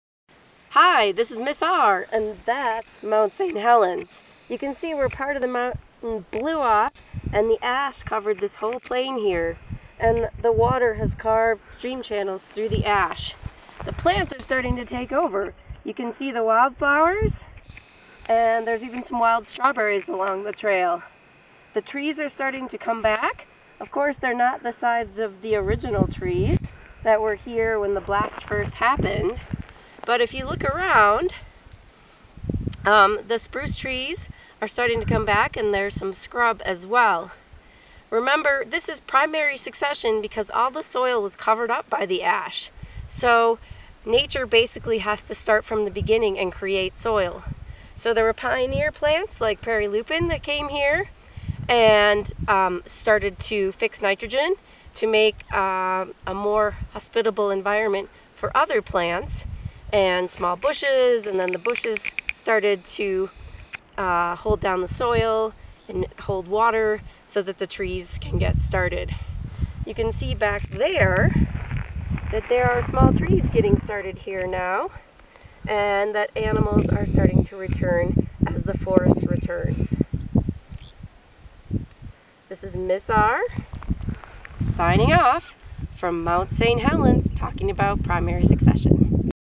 Mt. St. Helens Primary Succession
Ms. R talks about how the ecology of the ash plain at Mt. St. Helens is changing over time due to primary succession.